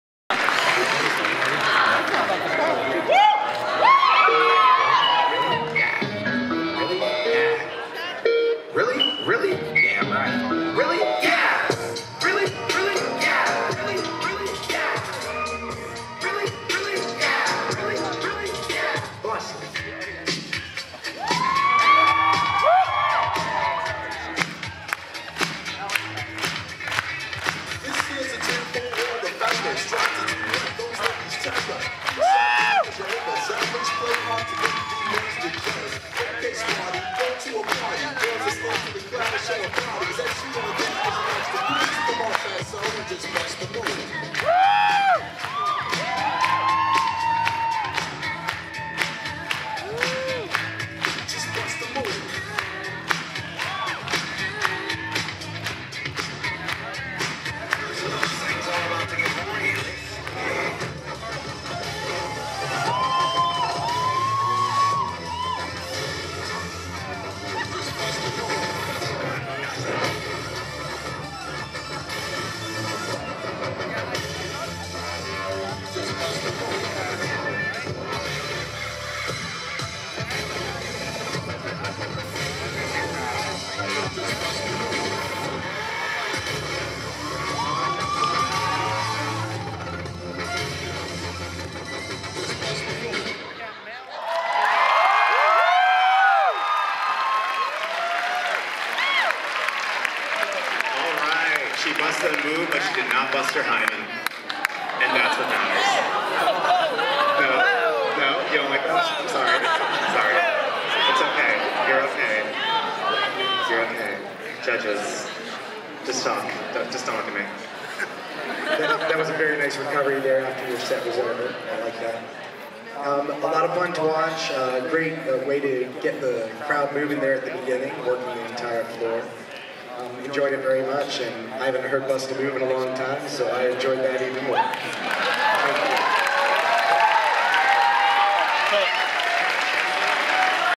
Tesla - FWA 2017 Dance Competition
Tesla owning it in the FWA 2017 dance competition.

Audio quality is potato. New mic coming in before Megaplex.